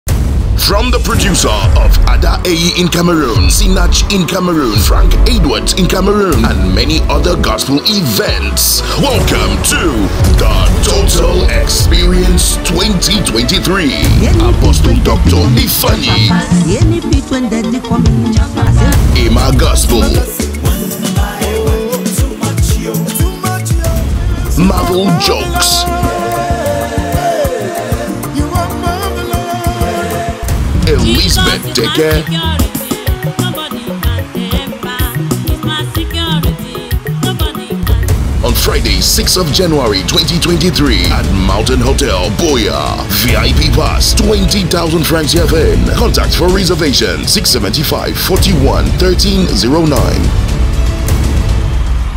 TOTAL EXPERIENCE 2023
Everything good for you in 2023 is set already 🔥
You are invited to an exclusive VIPgospelconcert experience, featuring top gospelartistes from 🇨🇲& 🇳🇬@Mountain Hotel Buea - 06/01/2023🙏🏾
(only 100 Seats available)